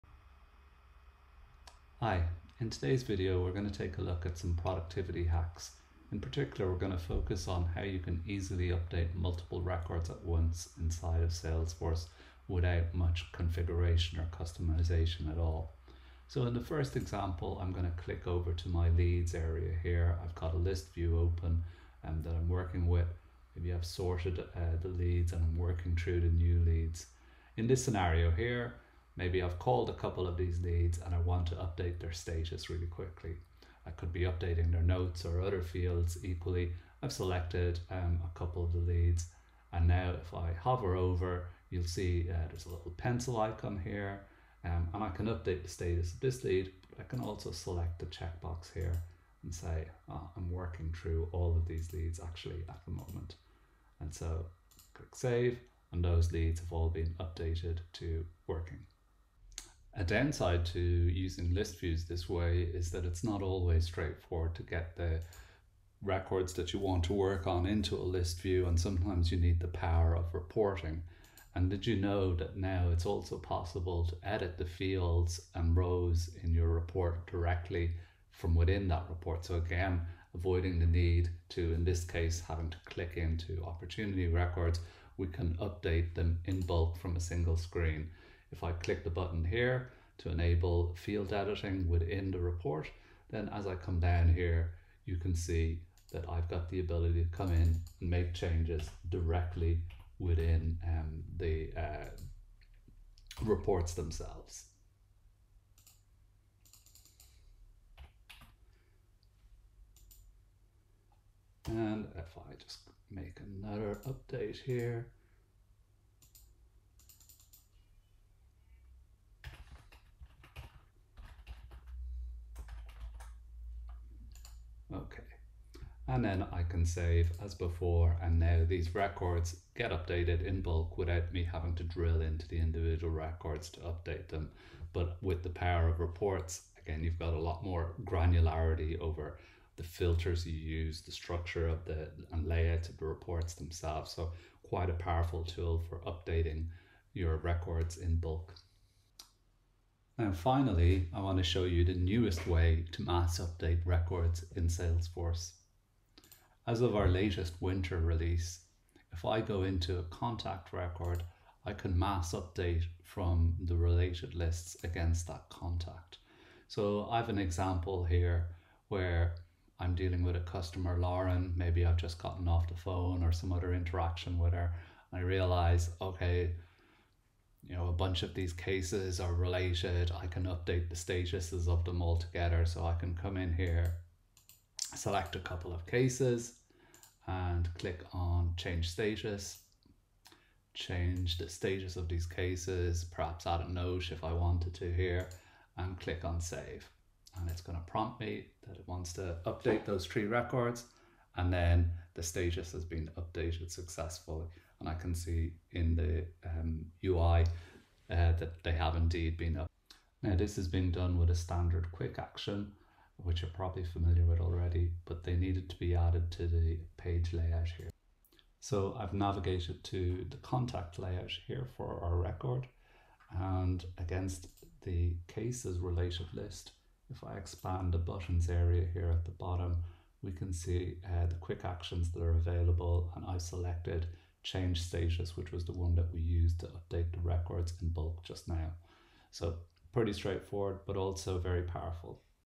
Mass Update / Bulk Update Records in Salesforce - Productivity Tips
Three simple ways to bulk update records in Salesforce to speed up productivity without having to click into each record individually.
We will look at mass updates using
- List Views
- Field Updates in Reports
- Mass Update Actions on Related Lists

Let me know in comments if you have other tips for bulk updating records